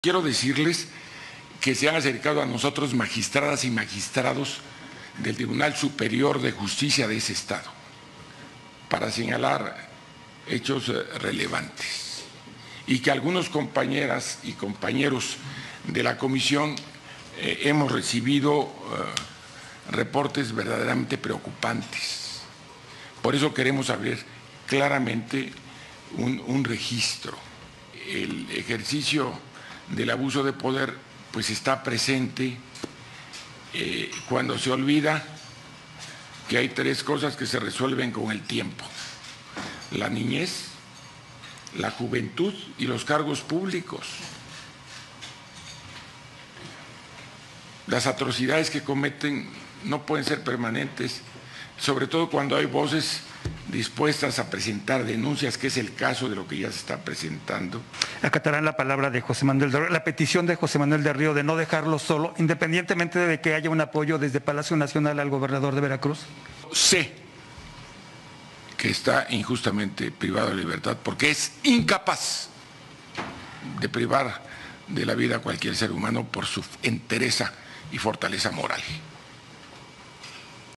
El abuso de poder está presente en Veracruz - Movimiento Ciudadano
A unos días de su creación, la comisión para investigar los abusos de poder en Veracruz ha recibido muchas... bastantes... quejas de irregularidades del Gobierno morenista de Cuitláhuac García Jiménez.